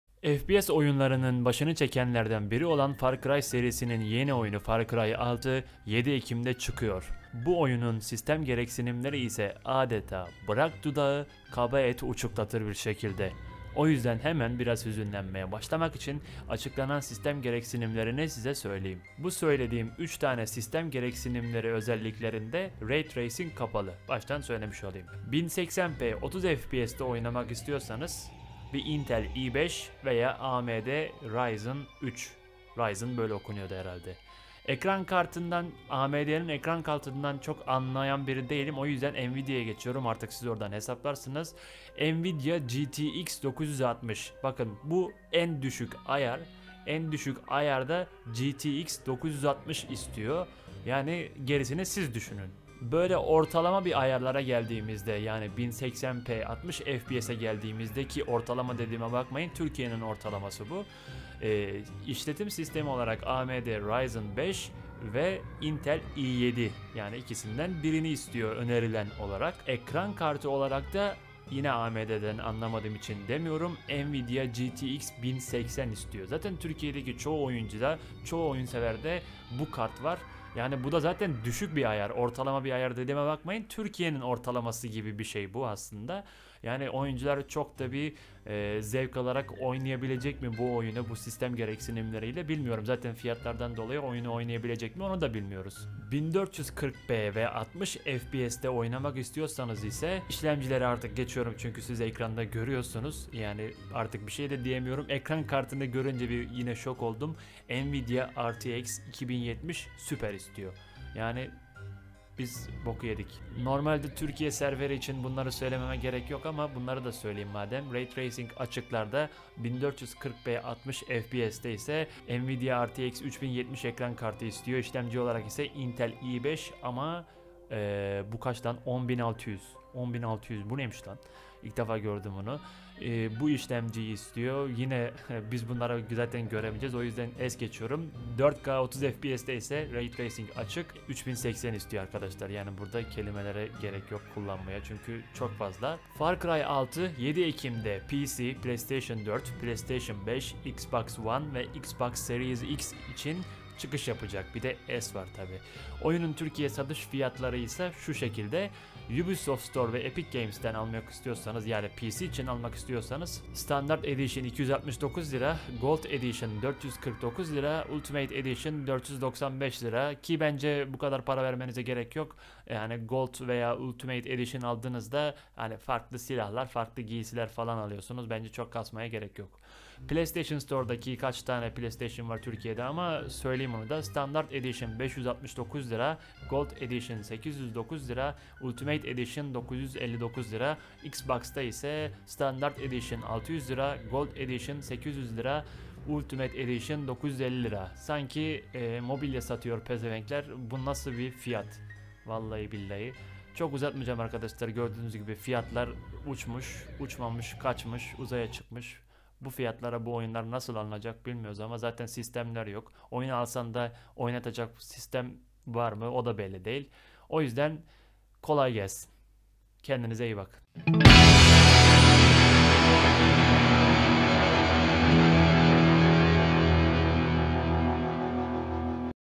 BU NASIL SİSTEM GEREKSİNİMLERİ? FAR CRY 6 SİSTEM GEREKSİNİMLERİ AÇIKLANDI !!! FAR CRY 6
Bu videoda Far Cry 6 oyununun açıklalan sistem gereksinimleri hakkında konuştum. İyi seyirler!!!